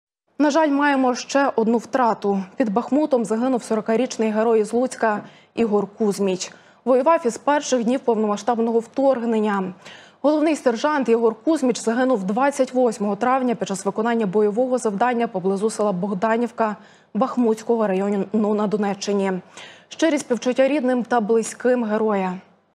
Під Бахмутом загинув 40-річний Герой з Луцька
На жаль, маємо ще одну втрату. Під Бахмутом загинув 40-річний Герой з Луцька Ігор Кузьміч. Воював з перших днів повномасштабного вторгнення. Головний сержант Ігор Кузьміч загинув 28 травня під час виконання бойового завдання поблизу села Богданівка Бахмутського району на Донеччині. Щирі співчуття рідним та близьким Героя.